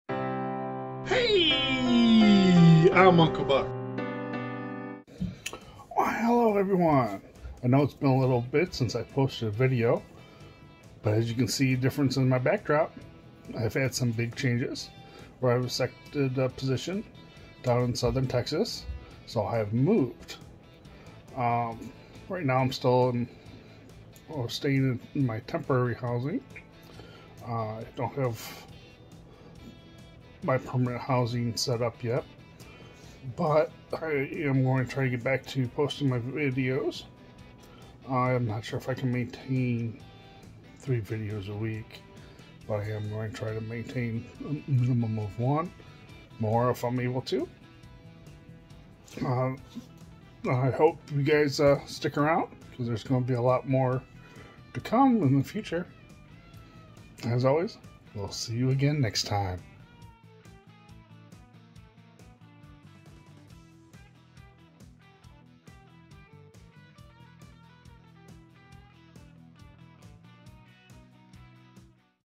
I moved to Texas
Hello everyone. Sorry it has been so long since I posted an video. Big changes happened in my life and happened quick. I accepted a job offer that I have been wishing for a long time and that was to leave Illinois and move to Southern Texas. I am so excited.